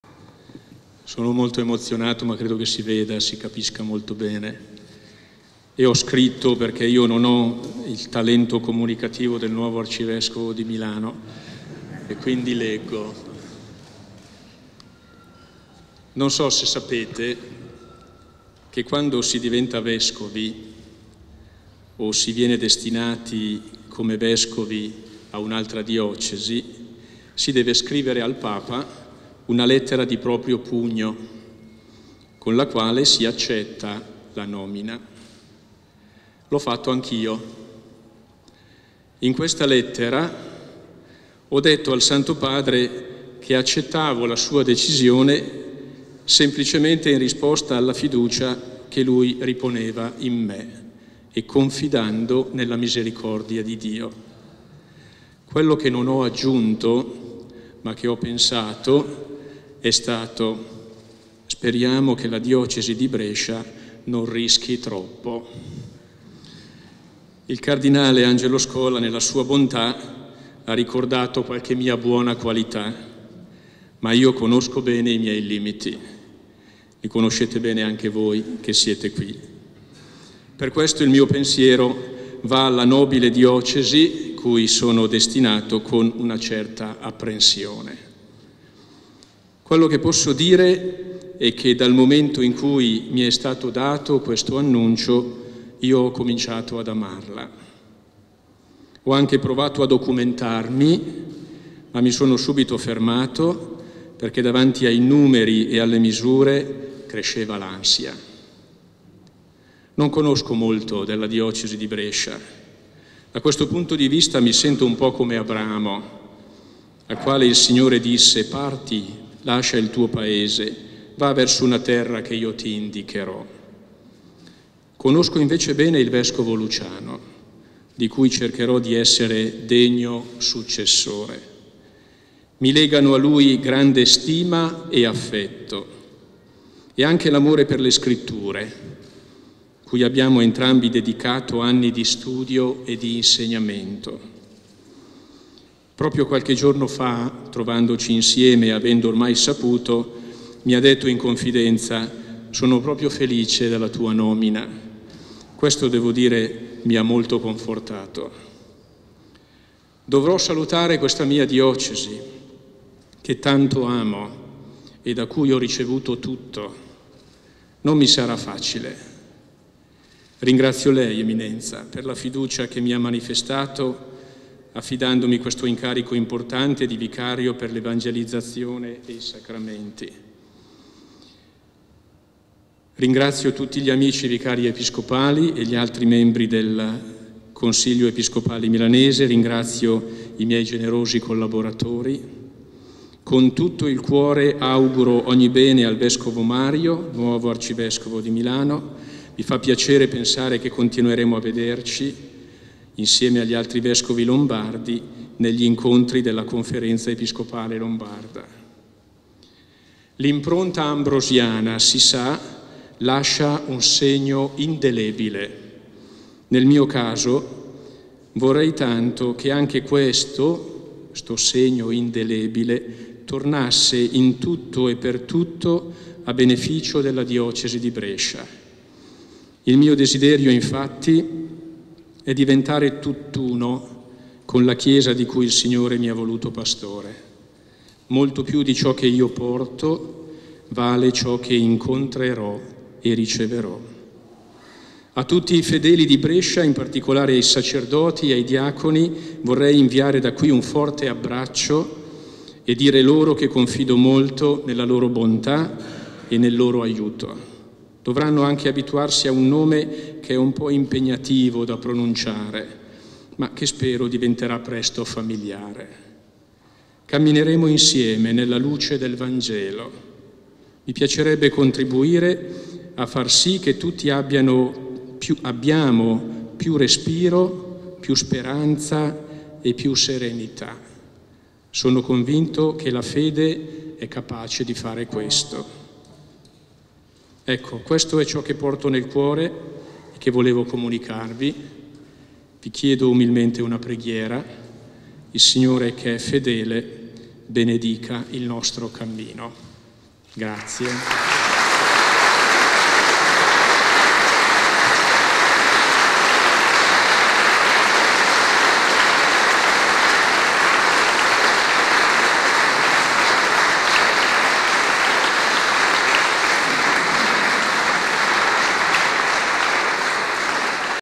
Monsignor Pierantonio Tremolada nuovo Vescovo di Brescia - il suo discorso
Monsignor Pierantonio Tremolada nuovo Vescovo di Brescia

La nomina da parte di papa Francesco comunicata dal cardinale Scola nella cappella arcivescovile della Curia di Milano